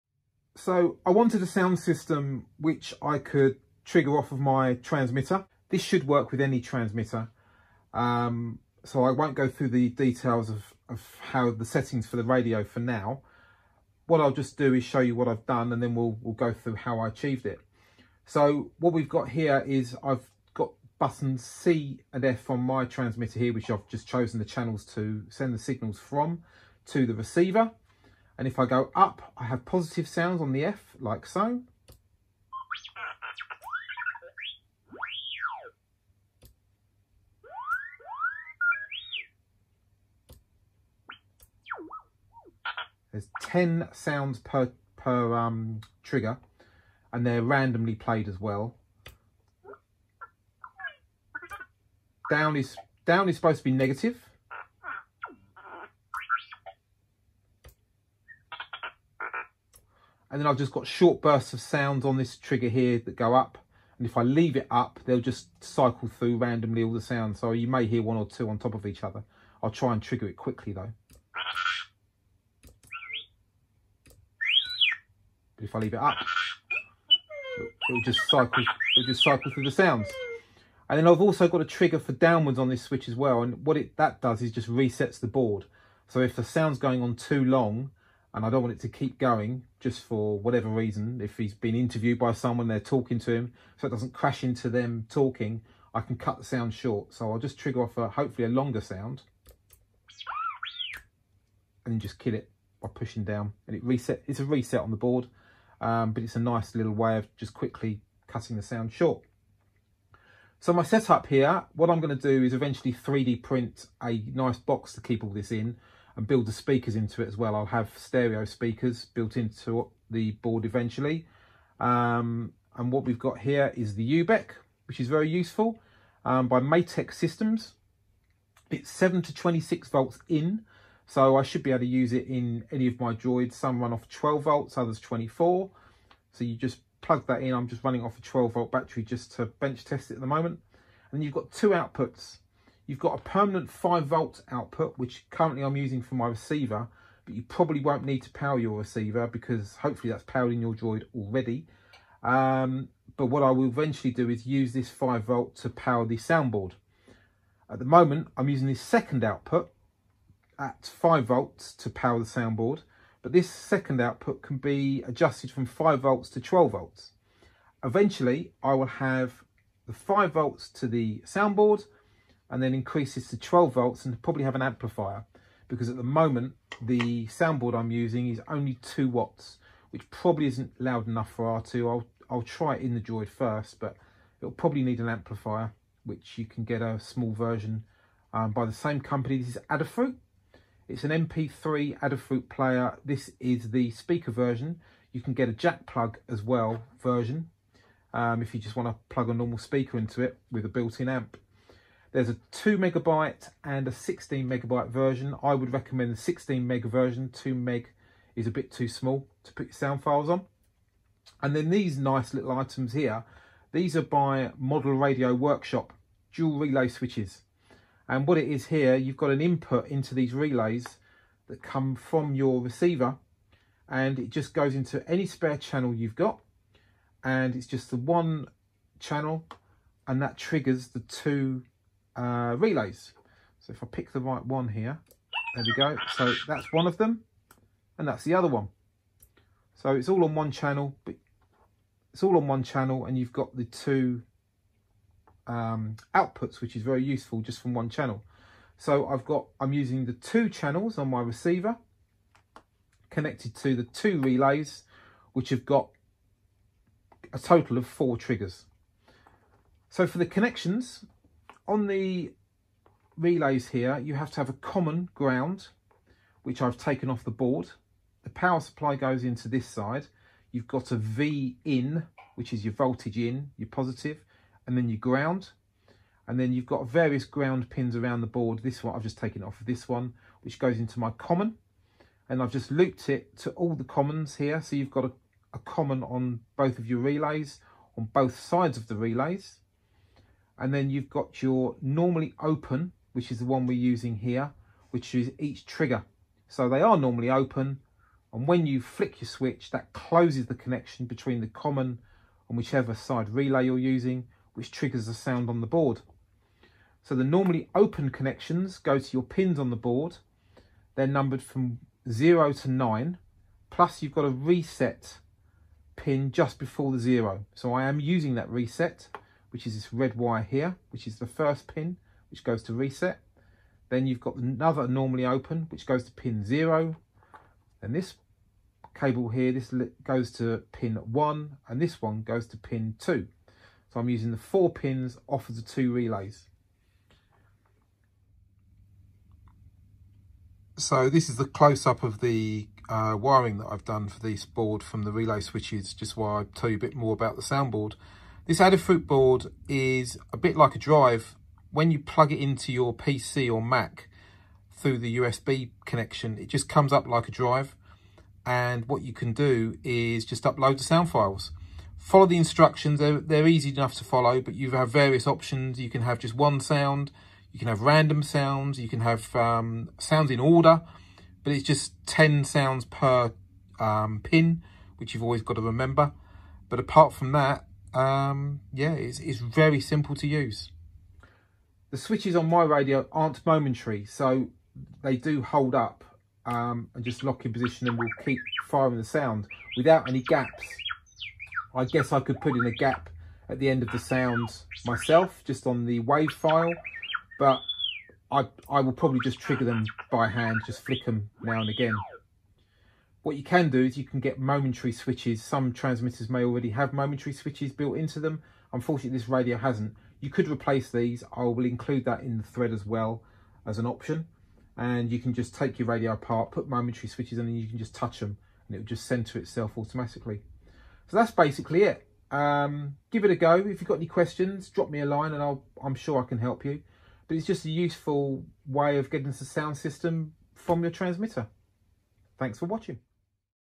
Sounds for Droids. How to build a sound module that can be triggered by a remote control transmitter
I wanted a sound system for my R2D2 that can easily be transferred from one droid to another.  For certain events the sound system sometimes works better outside of the droid so you can simply remove this system and have it placed somewhere else, yet still trigger it via remote control.

Equipment Used:
Spektrum DX9 Transmitter
Spektrum Receiver
12v Battery Pack

1x Adafruit Audio FX Sound Board - WAV/OGG Trigger – 16MB storage with 2x2W Amp

As many spare channels as you want to use (I use 2 in this tutorial) Dual Relay Controller Switch

Optional:

May work in other transmitters.